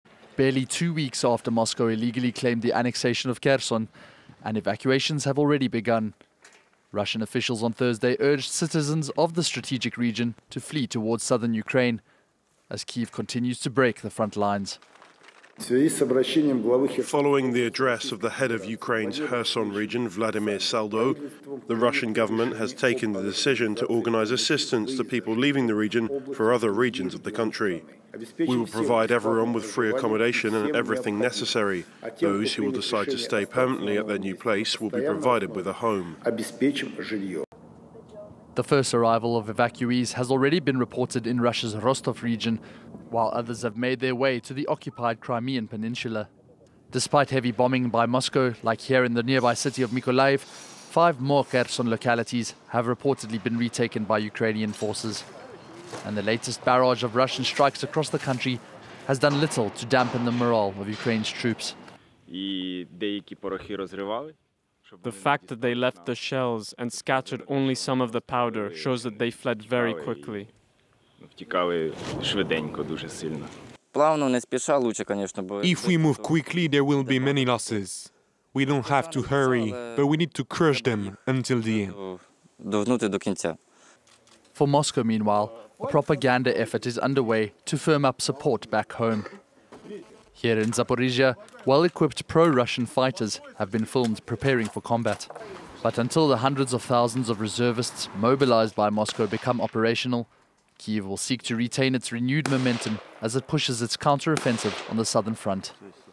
Russia, under pressure in southern Ukraine, captures villages in east • FRANCE 24 English
Russian-backed forces have made some advances in eastern Ukraine, Britain said on Friday, even as Moscow's hold weakens in the south, where a Russian-installed official has advised residents to flee a region Russia claims to have annexed.